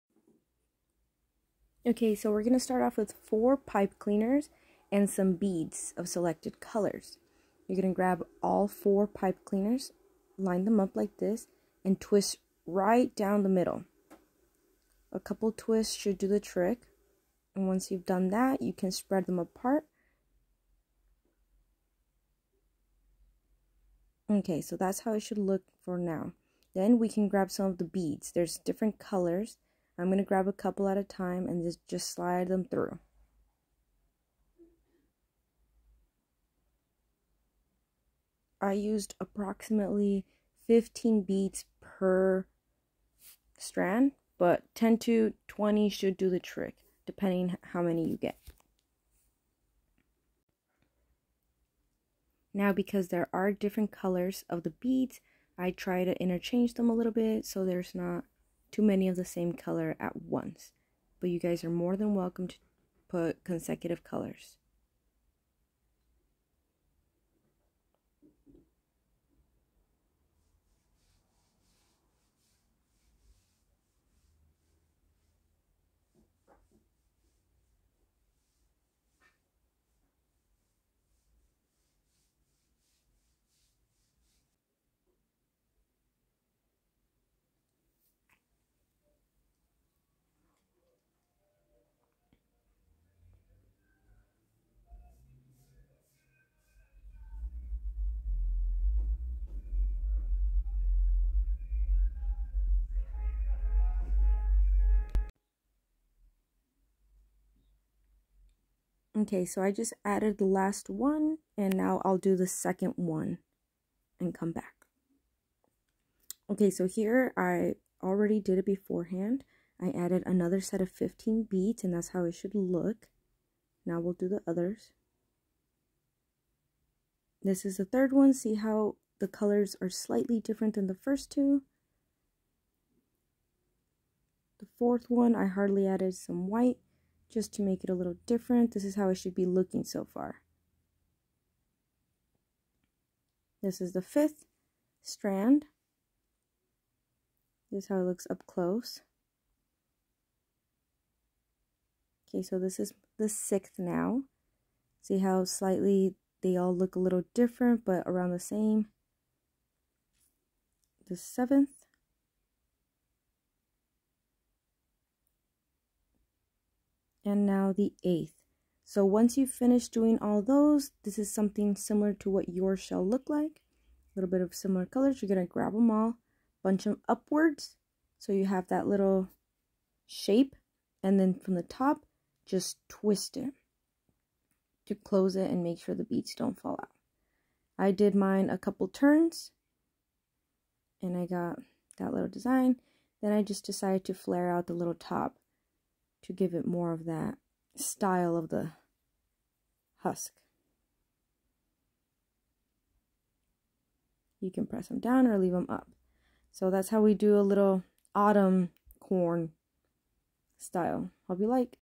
Flint Corn Craft
Making some flint corn decoration with pipe cleaners and beads.

4 pipe cleaners
plenty of autumn colored beads